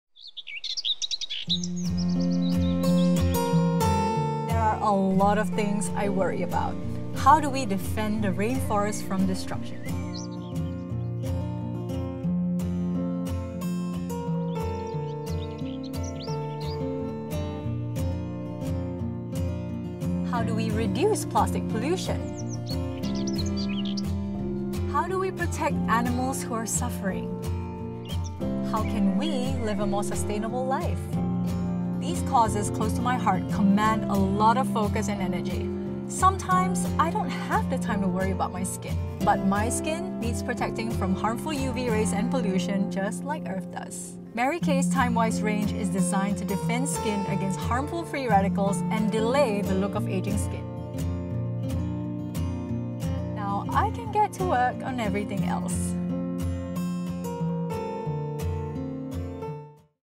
FEMALE x Mary Kay: Skincare Secrets With Melissa Tan
Model, TV host, and an advocate for sustainable green living, Melissa Tan (@melissatanlh) doesn't always have the time to worry about her skin. Thankfully she doesn't have to.

The Mary Kay TimeWise® Miracle Set 3D™ takes a three-dimensional approach to skin ageing by defending against free radicals, delaying the look of ageing with UVA/UVB protection, and delivering visible improvement. MaryKayMalaysia @marykaymysg

Credits:
Talent: Melissa Tan
Coordination: Yi-Di Ng
Art Direction: Stephanie Liaw
Videography: Ahmad Zulkamal
Hair & Makeup: Clara Siew
Stylist: Amalina Anuar
Venue: Free Tree Society